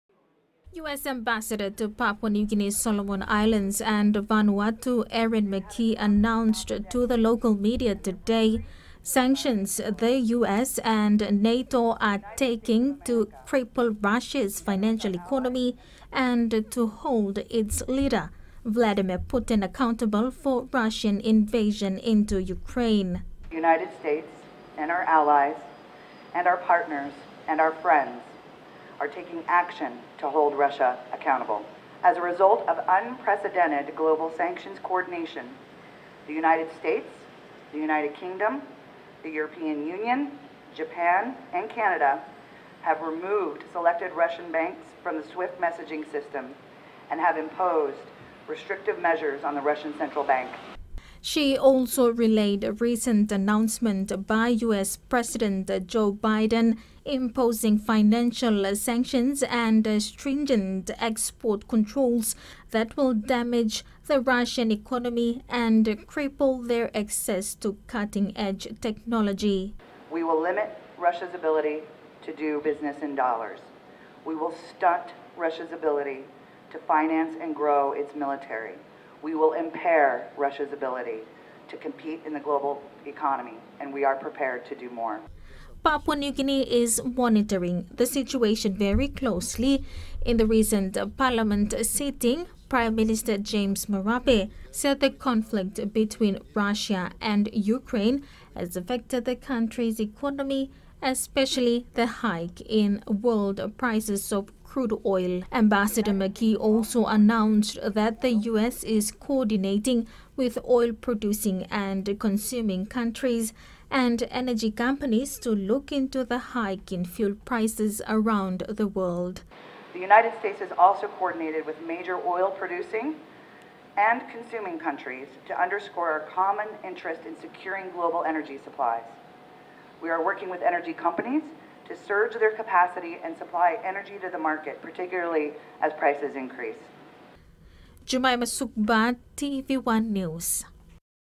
US Sanctions On Russia
As the world watches closely on the situation between Russia and Ukraine, the US has announced plans to cripple Russia’s financial economy.

US Ambassador to Papua New Guinea, Solomon Islands, and Vanuatu Erin McKee announced the sanctions to the PNG media this morning.
She said NATO and its friends are united more than ever.